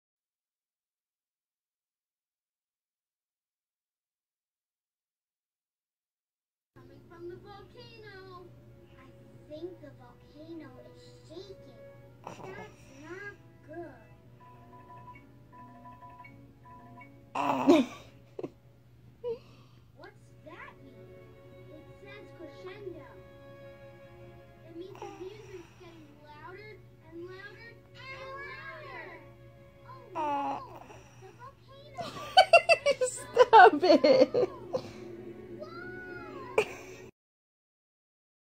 Vaikas negali nustoti žavėtis savo mama | Cute Baby Looking At Mother
Internetą užkariauja naujas žavingas video, kuriame vaikas negali nustoti žavėtis savo mama. Siūlome ir jums tai peržiūrėti.

Cute Baby Looking At Mother.